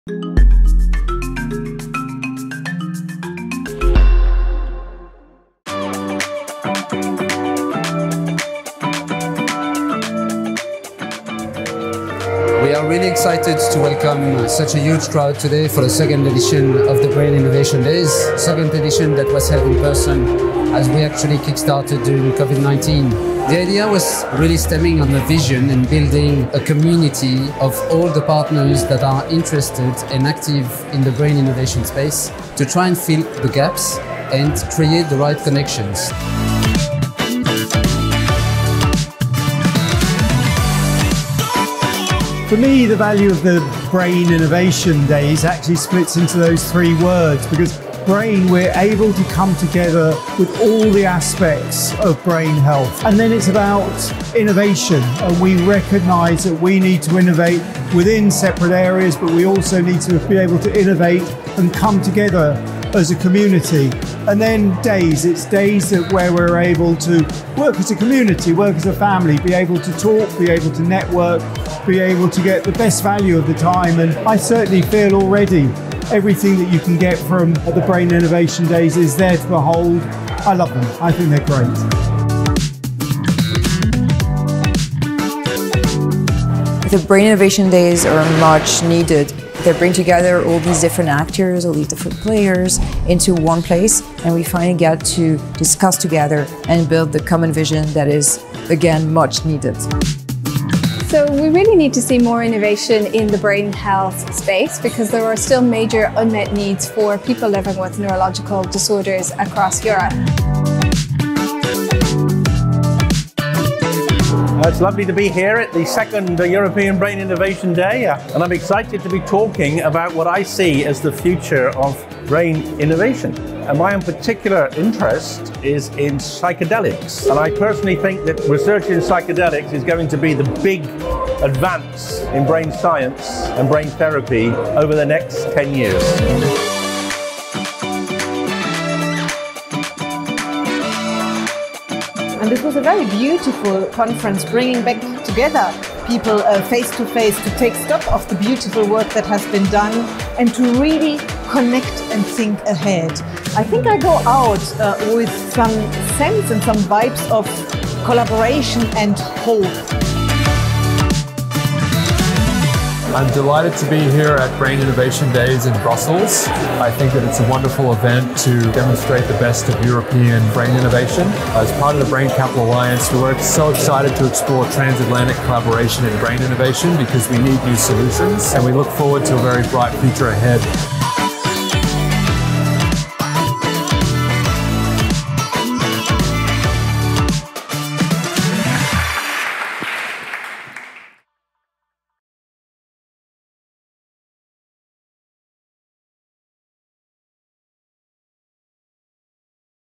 Brain Innovation Days 2022 - Official Aftermovie
On 11-12 October 2022, the European Brain Council was pleased to host its second in-person Brain Innovation Days in Brussels (Belgium). Held in conjunction with the Final Conference of the European Brain Research Area, the Days brought together the main circle of influence in the brain sector, with key players in the brain research and innovation field networking together: investors, researchers, clinicians, patient representatives, policymakers and more.